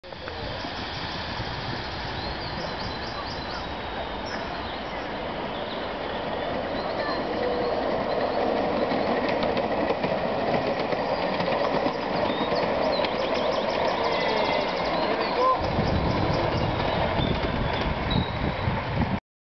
Ray and Speedy vid.AVI
A regular locomotive on the Goffs Park Railway is this Speedy. Today being run after a repair job to the backhead and fire-hole doors.